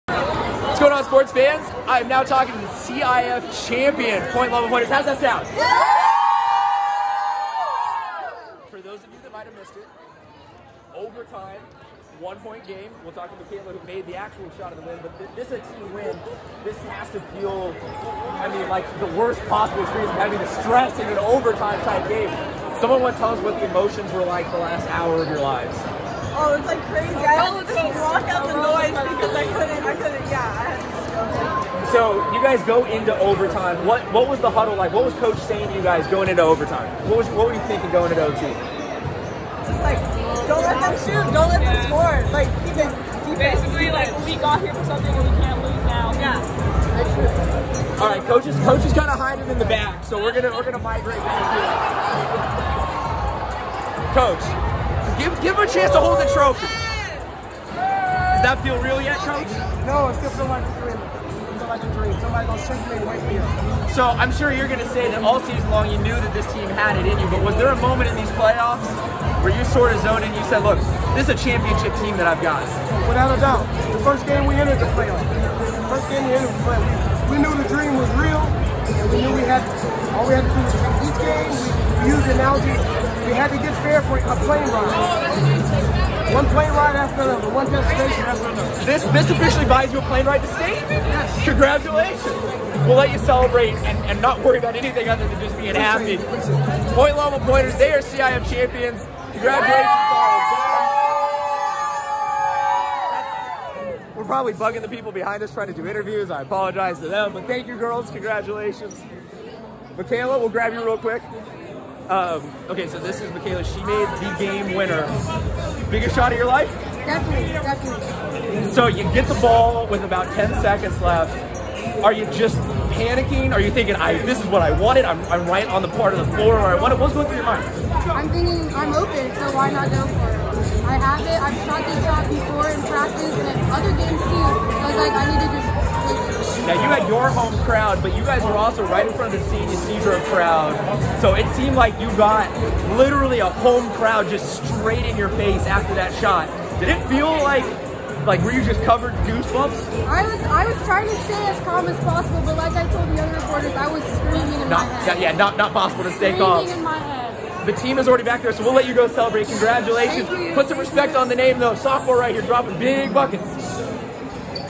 Post Game with Point Loma girls basketball
We speak to Point Loma girls basketball after they won the CIF D3 girls basketball championship in overtime thanks to a Mikaela Miles 3 pointer.